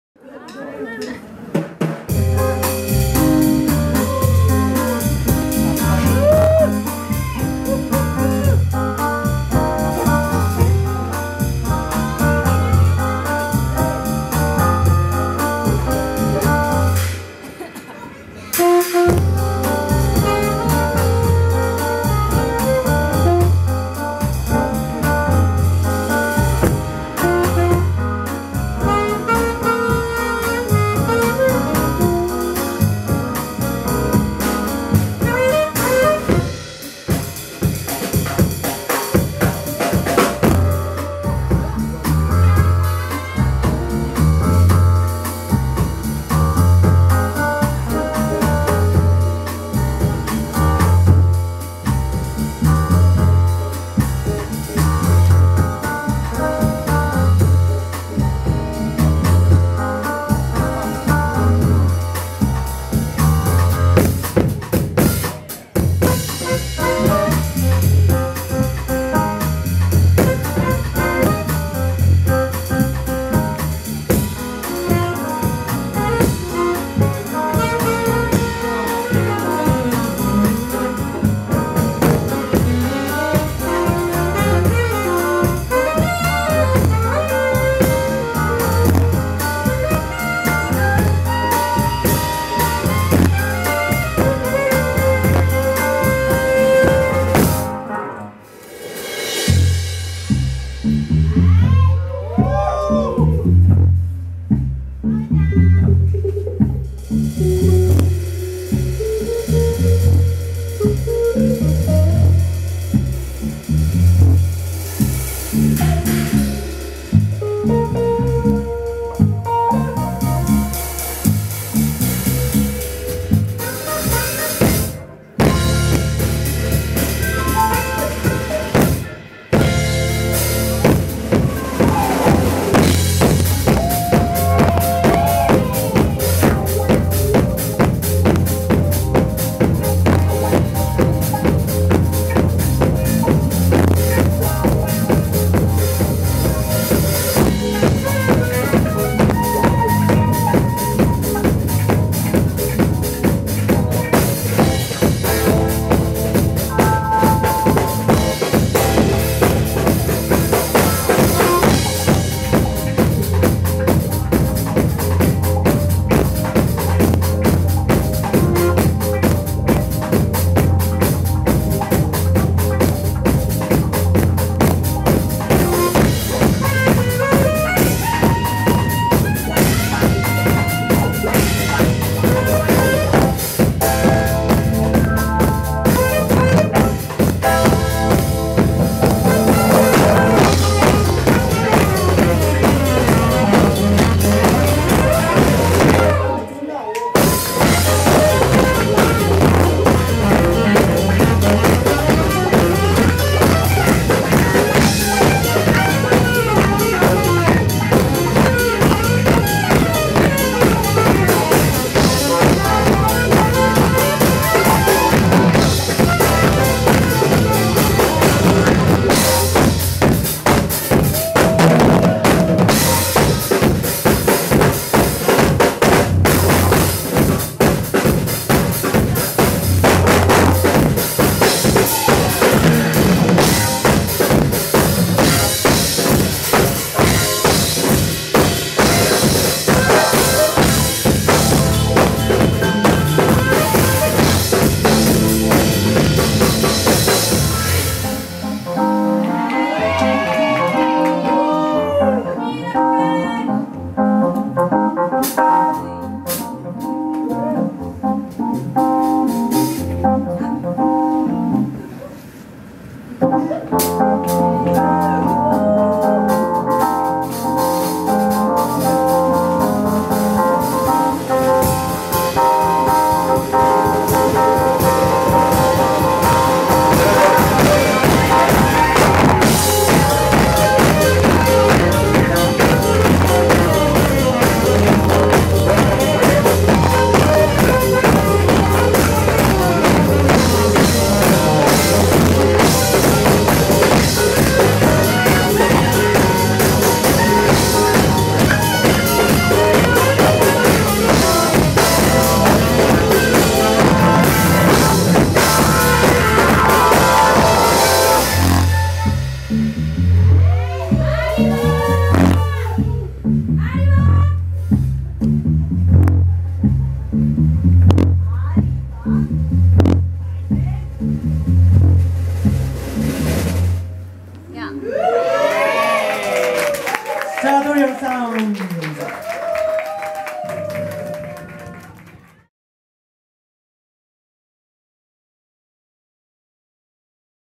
20131012 NyteGame SARTORIAL SOUNDS
2013/10/12 代々木 ANCE で行われた NyteGame での SARTORIAL SOUNDS さん達のパフォーマンスです。